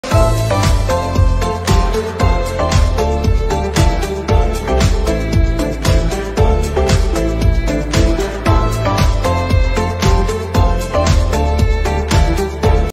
Designer Casagear Accent Tables | Free Shipping & Returns
Elevate your home décor with the Casagear Accent Table, combining style and functionality for any space. Its sleek design and durable build make it perfect for living rooms, bedrooms, or hallways. Enjoy free shipping sitewide and a 30-day free return policy, ensuring a risk-free purchase

  / casagear